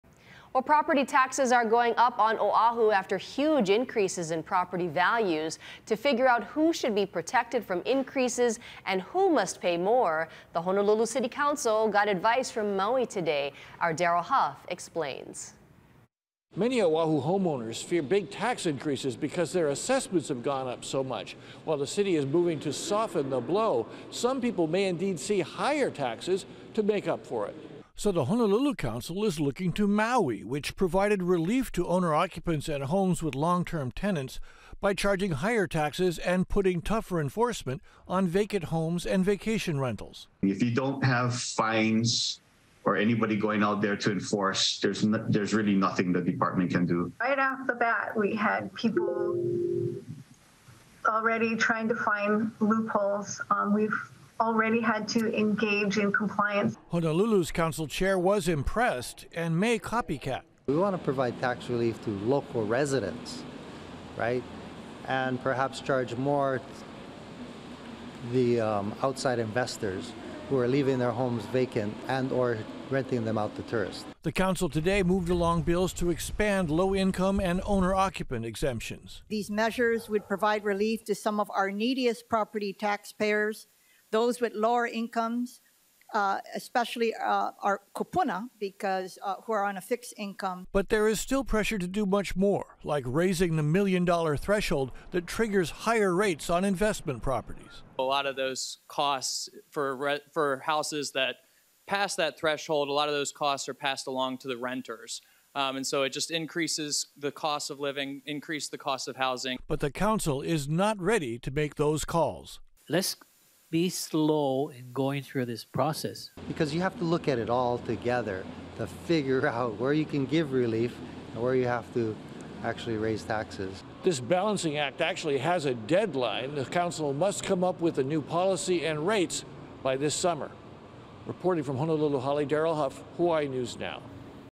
With property taxes set to rise, Honolulu seeks advice on how to protect low-income families
Property taxes are going up on Oahu after huge increases in property values.